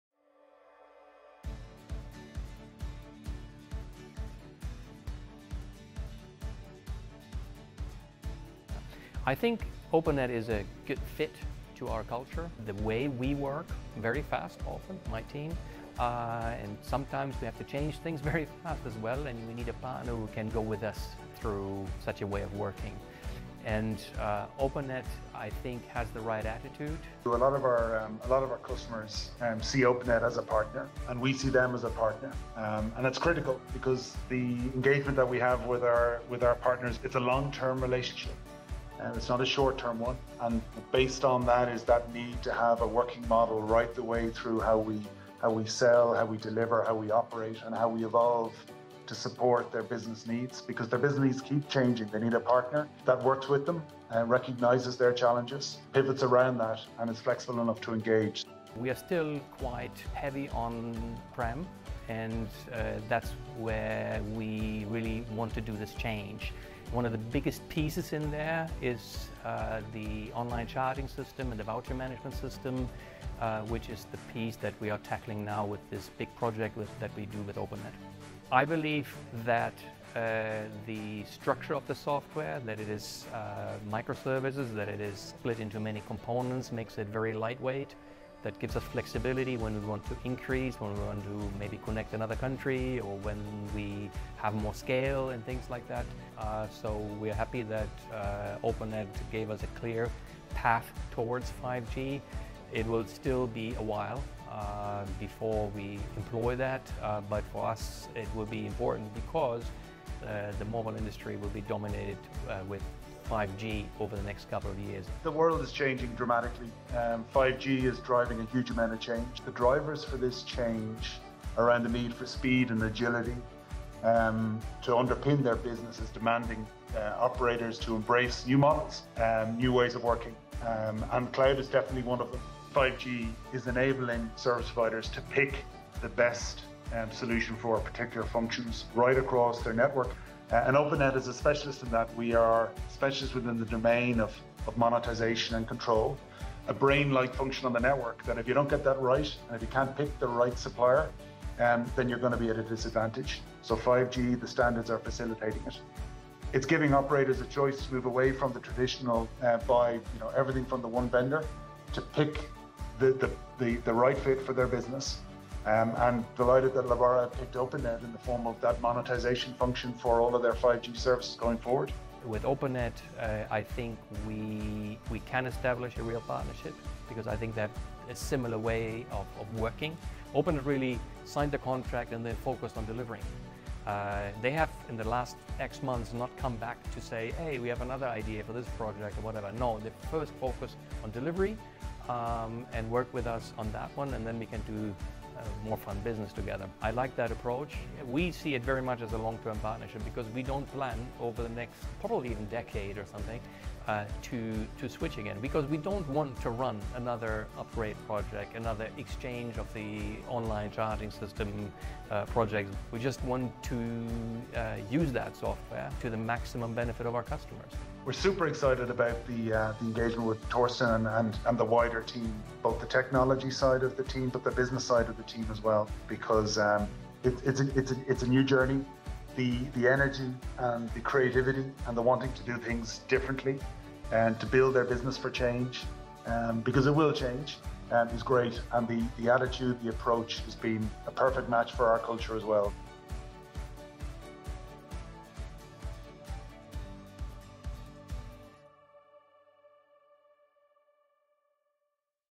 Lebara: In partnership with Openet
Openet is critical to Lebara's journey to the cloud. Lebara CTO Torsten Minkwitz and Openet's Paul Saunders tell us more.